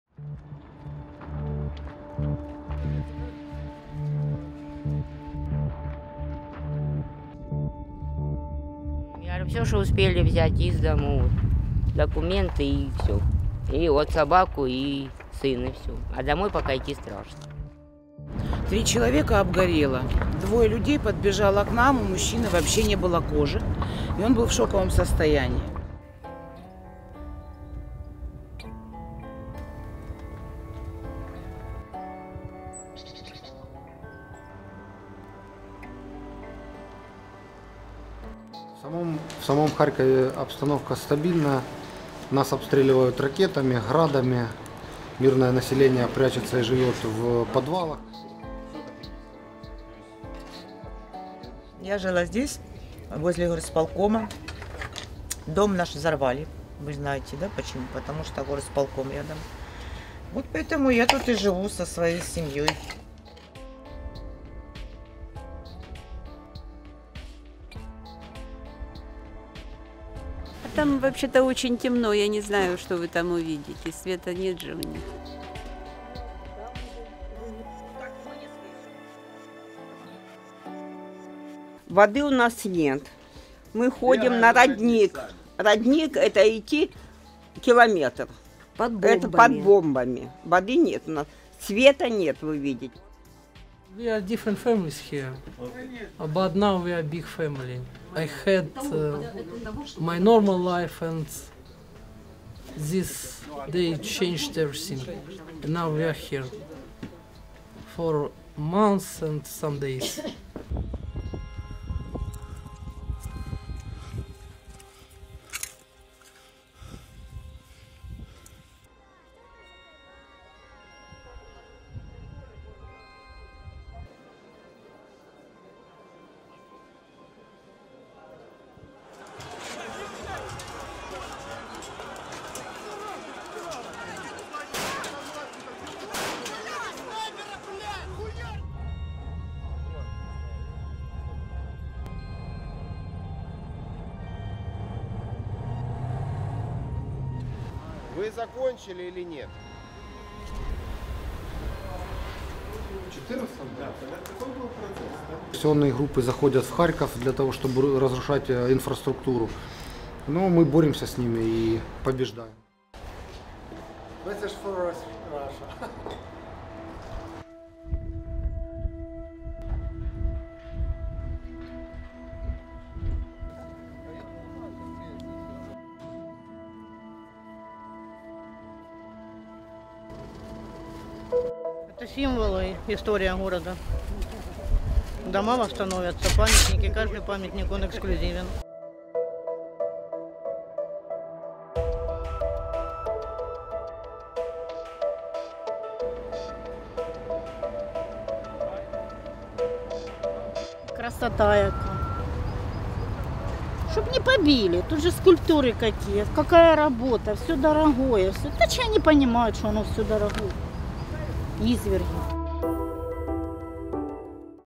Kharkiv: life under fierce Russian bombardment | AFP
With the Russian army camped at the city's gates, the days all seem the same in Kharkiv: rockets and missiles fall regularly on Ukraine's second city. More than 1.5 million live in the majority Russian-speaking city, lying at the confluence of three rivers, located around 40 kilometres from the Russian border. On the invasion's first day on February 24, Russian forces descended on the city, penetrating the suburbs, but were then pushed back to Kharkiv's northern and eastern outskirts. Since then, Russian artillery has been striking day and night, especially in the north and east of the city.